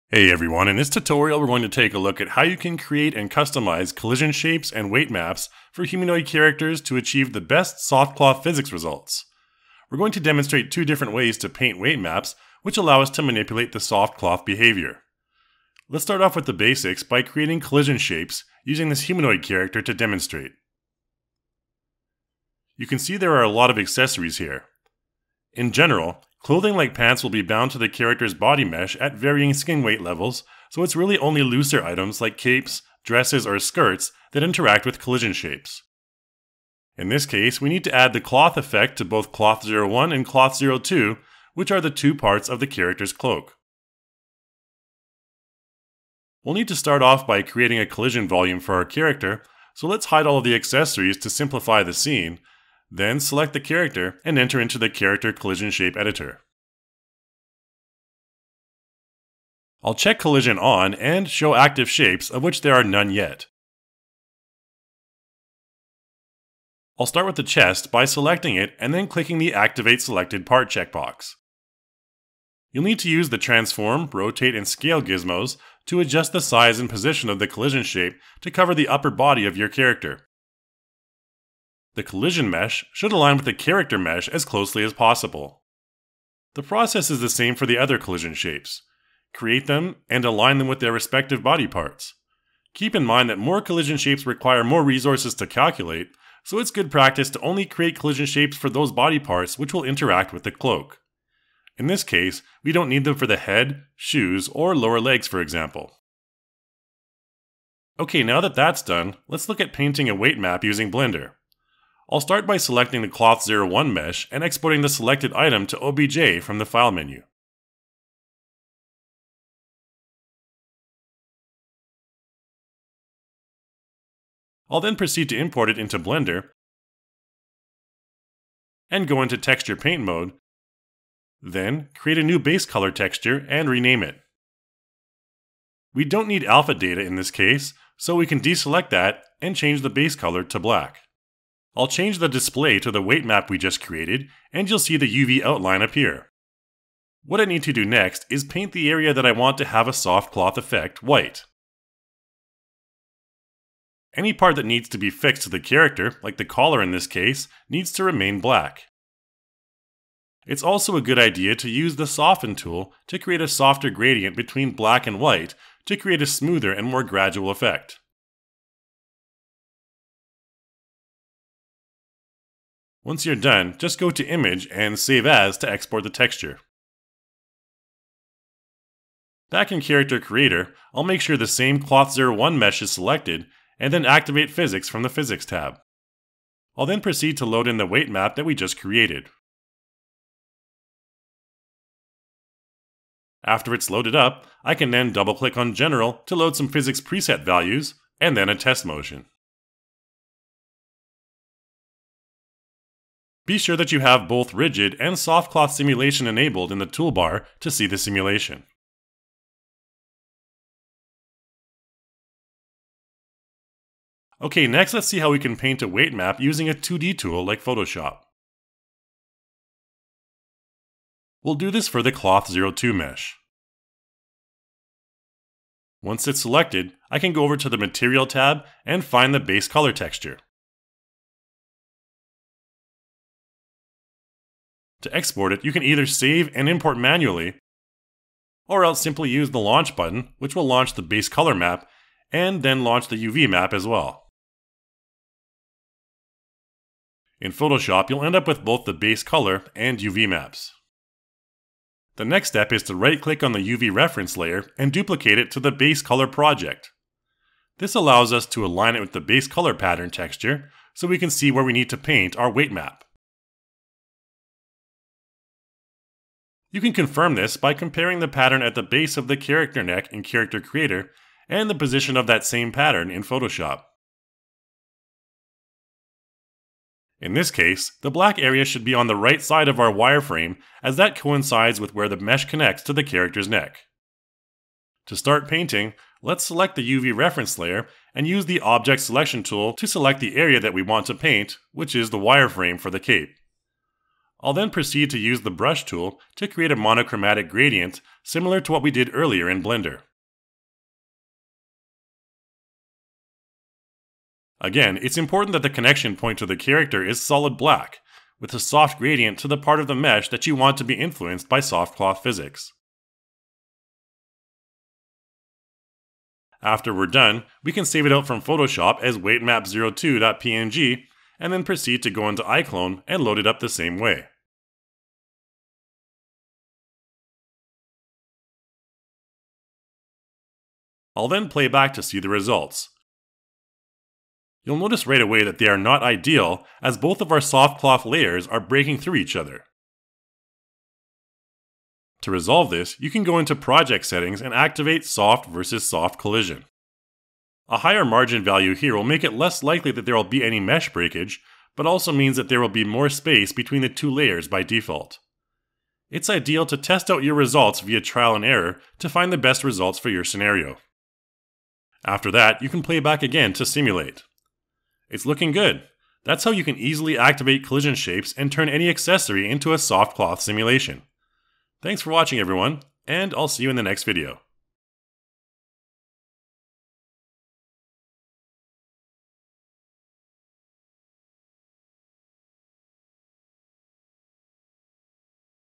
Character Creator 4 Tutorial - Creating Collision Shapes & Soft Cloth for Humanoid Characters
Soft cloth physics are a neat feature with Character Creator and iClone that allows you to quickly animate realistic cloth results on things like dresses, capes, cloaks, ribbons and skirts.  In this tutorial, you’ll learn about how you can activate the collision shapes on your characters and quickly generate weight maps in both Blender and Photoshop.

00:00 Intro
00:15 Creating Collision Shapes
01:50 Painting Weight Maps with Blender
03:35 Painting Weight Maps with Photoshop
05:40 Soft vs Soft Collision

✦ONLINE HELP
[Character Creator]